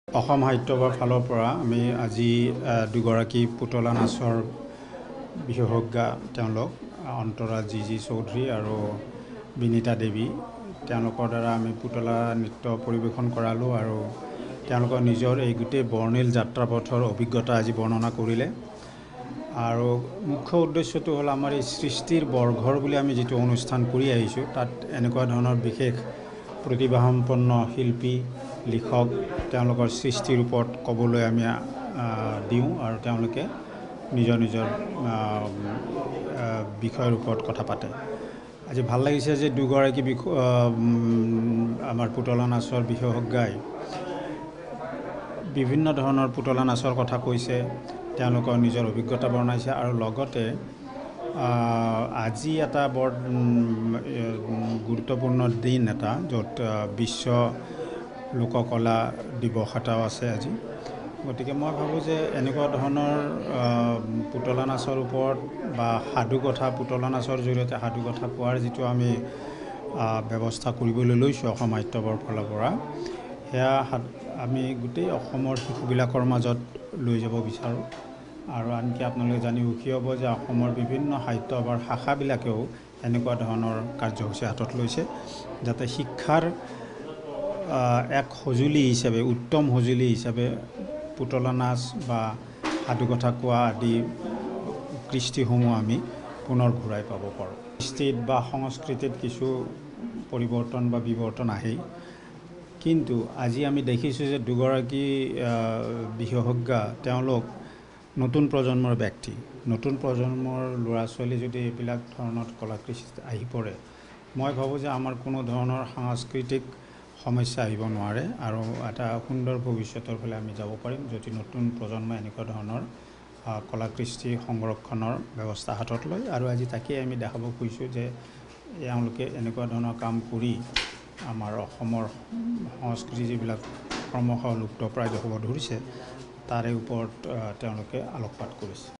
A special 'doll dance' programme of Assam Sahitya Sabha!
A special 'doll dance' programme of Assam Sahitya SabhaWatch the full clip to know more about this story.